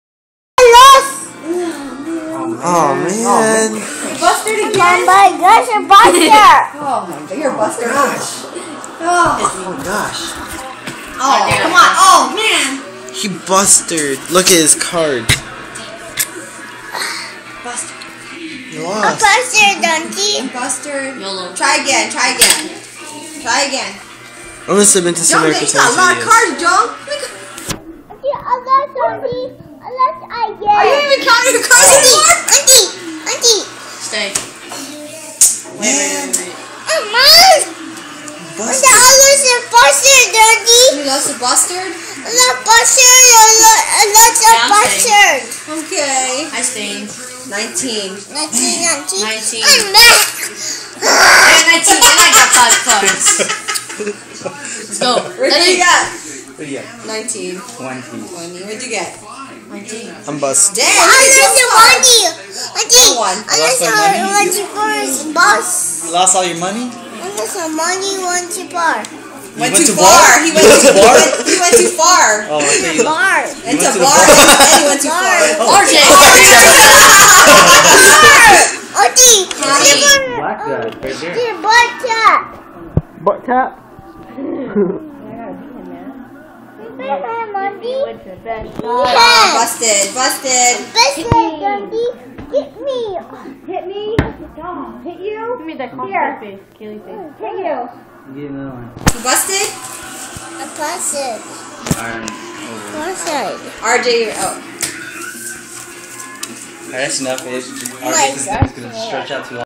I'm Bustard
Baby Aidan plays Blackjack.